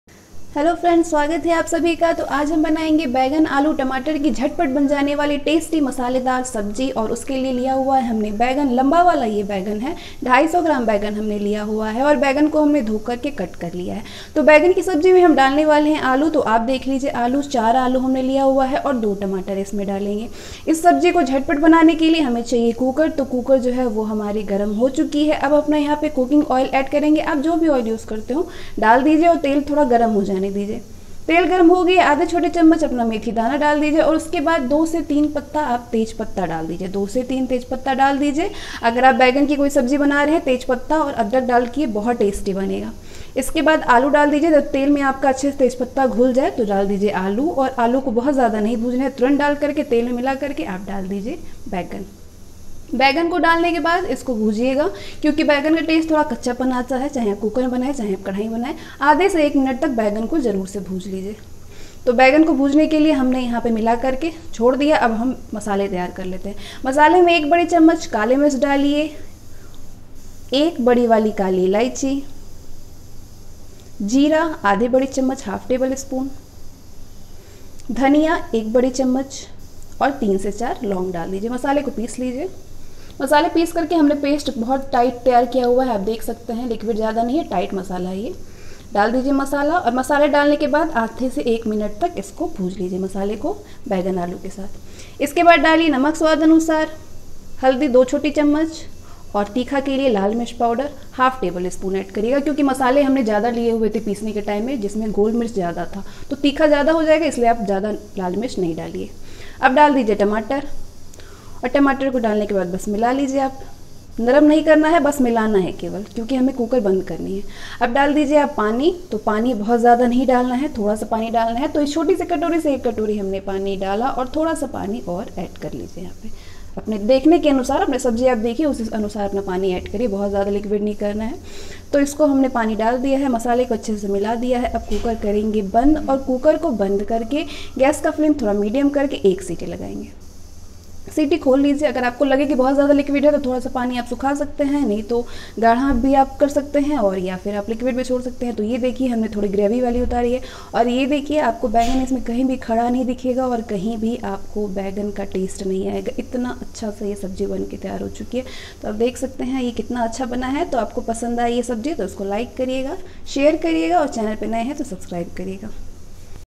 मसालेदार बैंगन आलू झटपट बनाइए बिहारी तरीके से/baigan aaloo ki masaledar sabji
ingredients list;
eggplant/ brinjals-250g
potatoes-4
tomato-2
black cardemom-1
cumin-1/2 tbsp